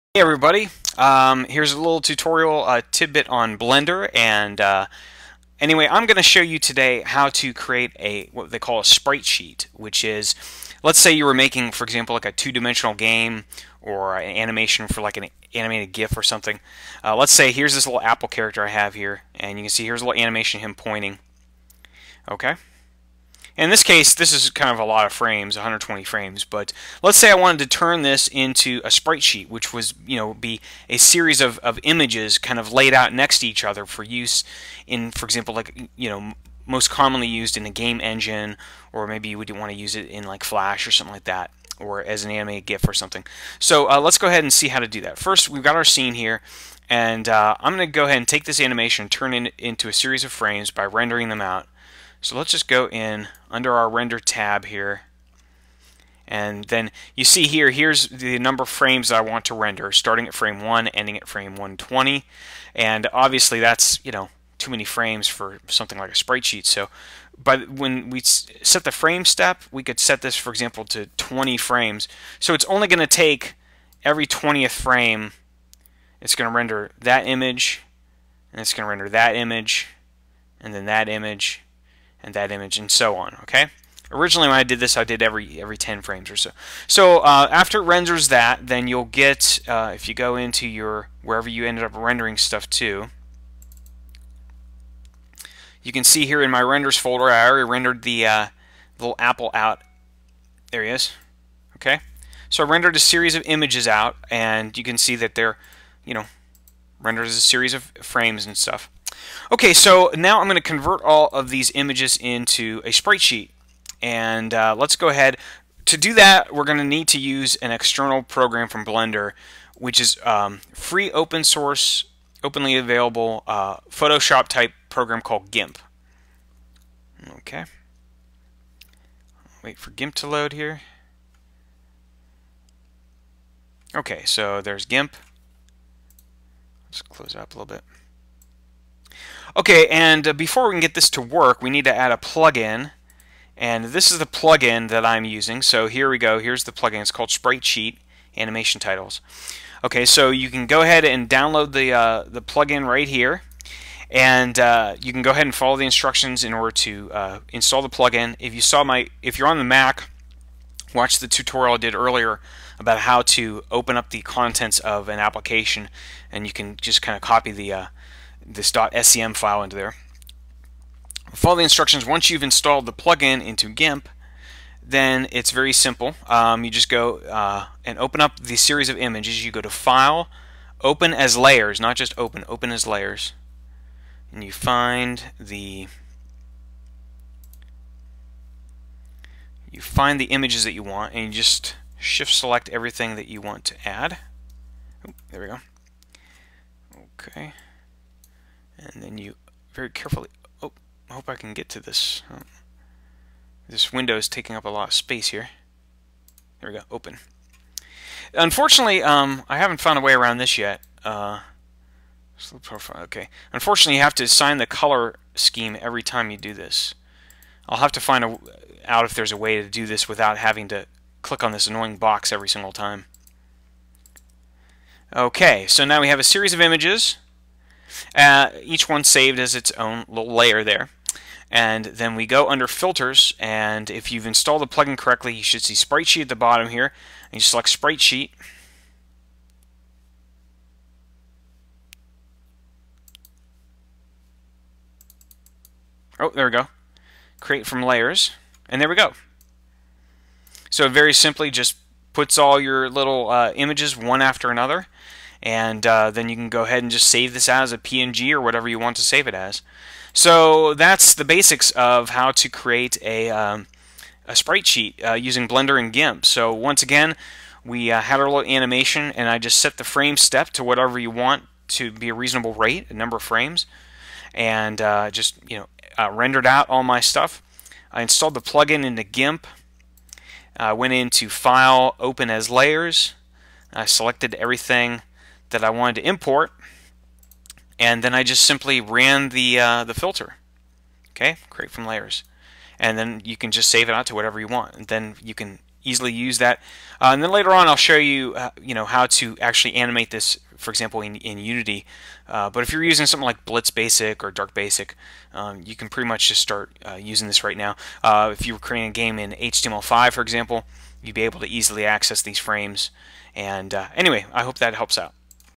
Blender-Creating Sprite Sheets
This tutorial explains how to  create a sprite sheet for use in 2D games using Blender and Gimp.